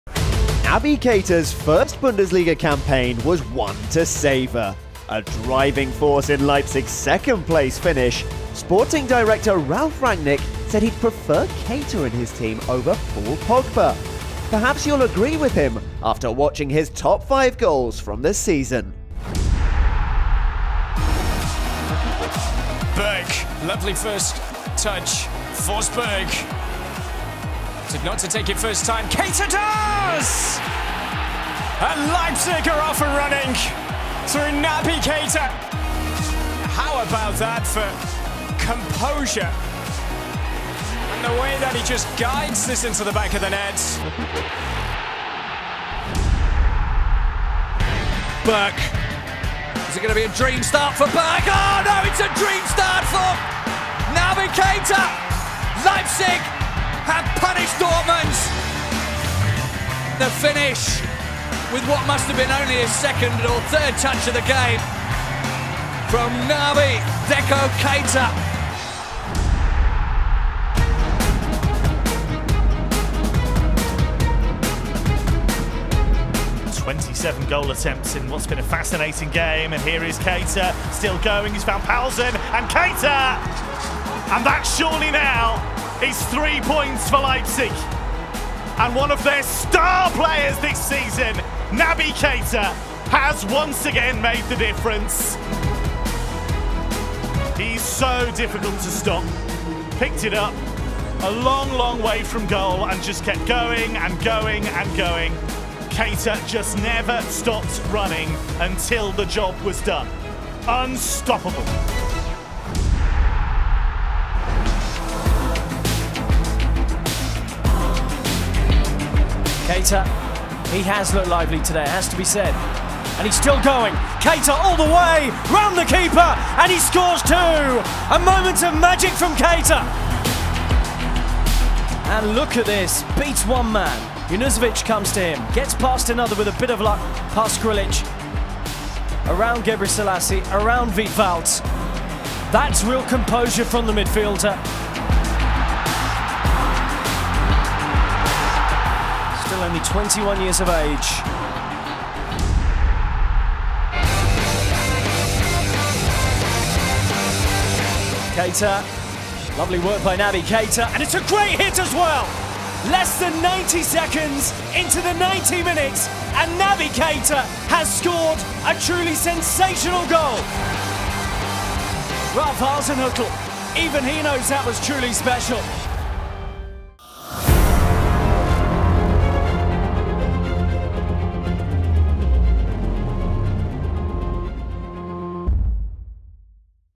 Naby Keita - Top 5 Goals - 2016/17 Season
Naby Keita enjoyed a sublime first season in the Bundesliga with RB Leipzig.

The Guinean star cracked in eight goals for Ralph Hasenhüttl's side as they finished second in their maiden top flight campaign - enjoy his finest five strikes from the 2016/17 season here.